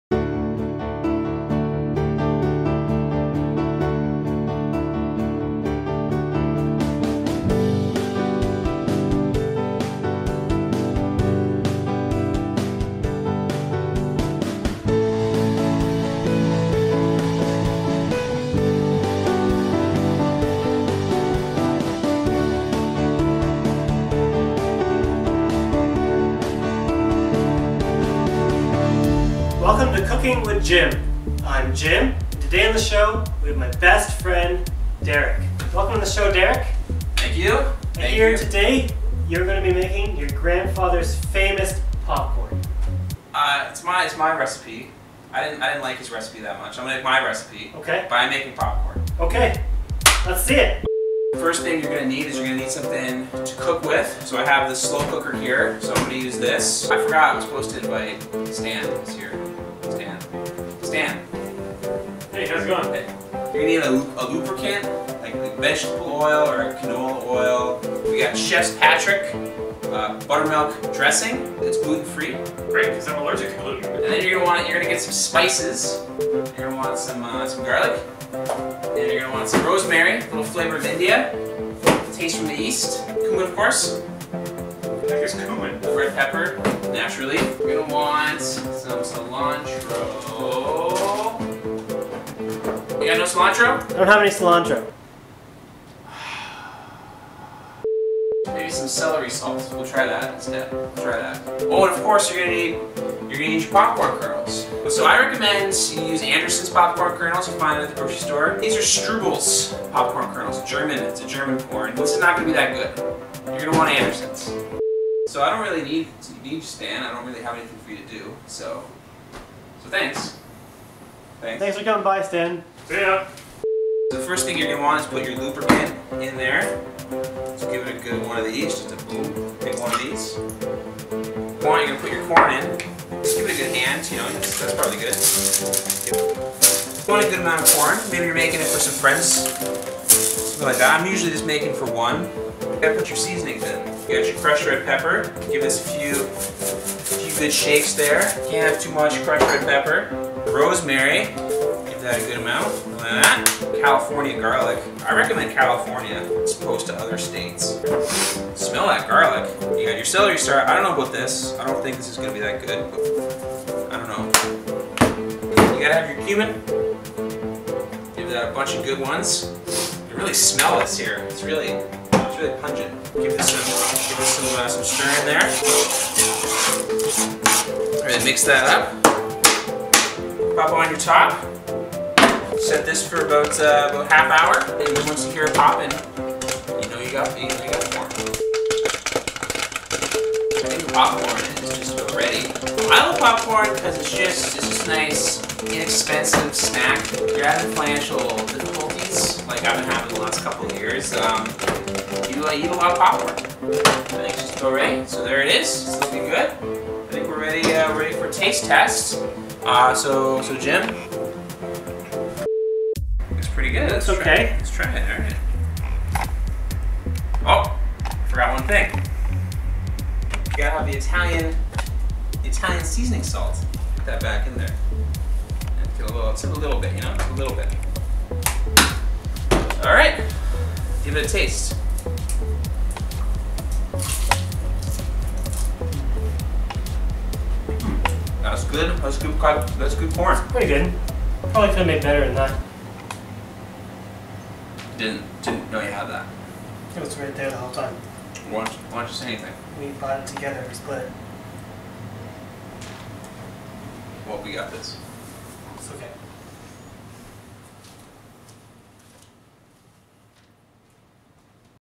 World's Best Popcorn! - Jim's Kitchen
Derek brings his famous popcorn recipe to Jim's Kitchen. This is a simple popcorn recipe that can be enjoyed by all. Popcorn is a low calorie snack that is extremely cheap and easy to make. All you will need is a lubricant of some kind, popcorn (Anderson's preferred), and a few spices. Please enjoy!

Let Jim know what you want him and his expert team to cook next!